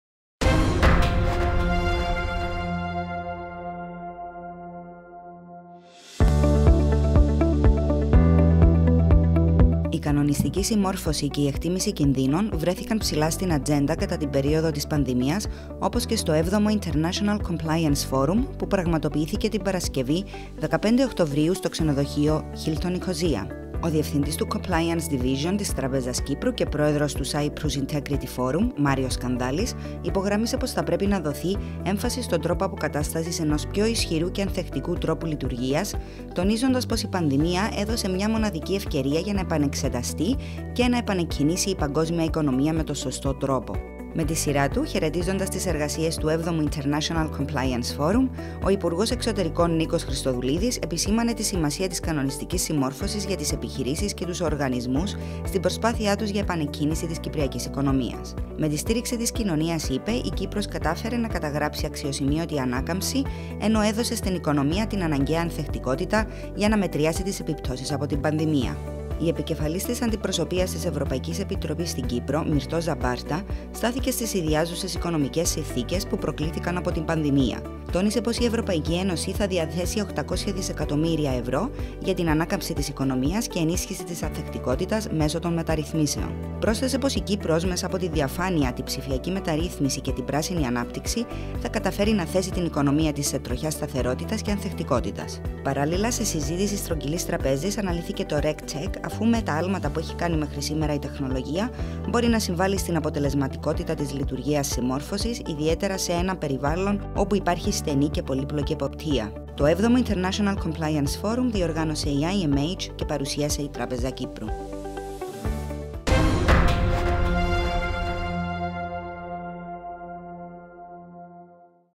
7o International Compliance Forum: Επανεξέταση και επανεκκίνηση στην οικονομία
Η κανονιστική συμμόρφωση και η εκτίμηση κινδύνων βρέθηκαν ψηλά στην ατζέντα κατά την περίοδο της πανδημίας όπως και στο 7o International Compliance Forum που πραγματοποιήθηκε την Παρασκευή 15 Οκτωβρίου, στο ξενοδοχείο Hilton Nicosia.